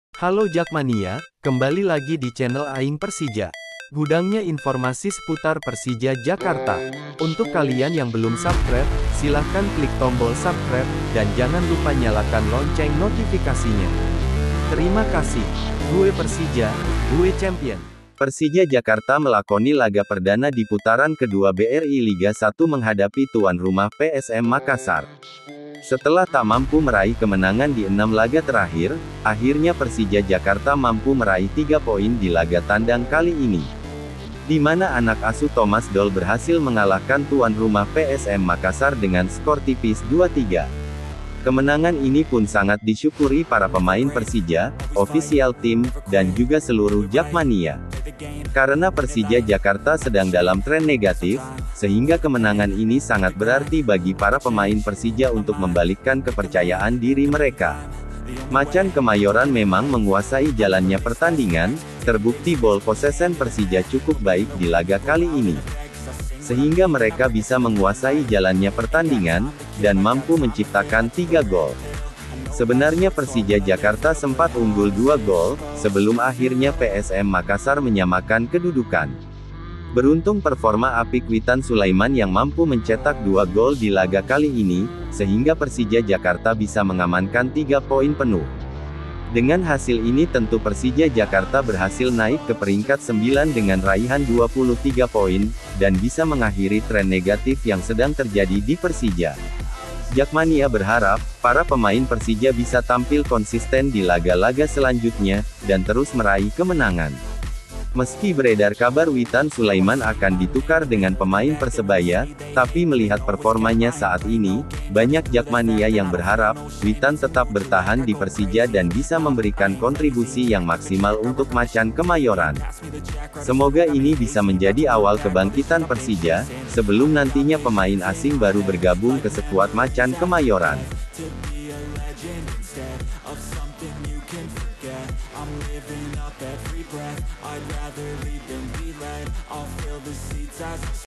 Brace Witan Sulaiman Bawa Persija Putus Tren Negatif ❗Akhirnya Menang Jak🤩
Persija Jakarta melakoni laga perdana di putaran kedua BRI Liga 1 menghadapi tuan rumah PSM Makassar.

setelah tak mampu meraih kemenangan di 6 laga terakhir,akhirnya Persija Jakarta mampu meraih tiga poin di laga tandang kali ini.

di mana anak asuh Thomas doll berhasil mengalahkan tuan rumah PSM Makassar dengan skor tipis 2 3.

kemenangan ini pun sangat disyukuri para pemain Persija,official tim,dan juga seluruh Jakmania.

karena Persija Jakarta sedang dalam tren negatif,sehingga Kemenangan ini sangat berarti bagi para pemain Persija untuk membalikkan kepercayaan diri mereka.

Macan Kemayoran memang menguasai jalannya pertandingan, terbukti ball posesen Persija cukup baik di laga kali ini.

sehingga mereka bisa menguasai jalannya pertandingan,dan mampu menciptakan tiga goll.

sebenarnya Persija Jakarta sempat unggul dua gol,sebelum akhirnya PSM Makassar menyamakan kedudukan.

beruntung performa apik witan Sulaiman yang mampu mencetak dua goll di laga kali ini,sehingga Persija Jakarta bisa mengamankan tiga poin penuh.

dengan hasil ini tentu Persija Jakarta berhasil naik ke peringkat 9 dengan Raihan 23 poin,dan bisa mengakhiri tren negatif yang sedang terjadi di Persija.

Jakmania berharap,para pemain Persija bisa tampil konsisten di laga-laga selanjutnya, dan terus meraih kemenangan.

meski beredar kabar witan Sulaiman akan ditukar dengan pemain Persebaya,tapi melihat performanya saat ini,banyak Jakmania yang berharap,witan tetap bertahan di Persija dan bisa memberikan kontribusi yang maksimal untuk Macan Kemayoran.

Semoga ini bisa menjadi awal kebangkitan Persija,sebelum nantinya pemain asing baru bergabung ke sekuat Macan Kemayoran.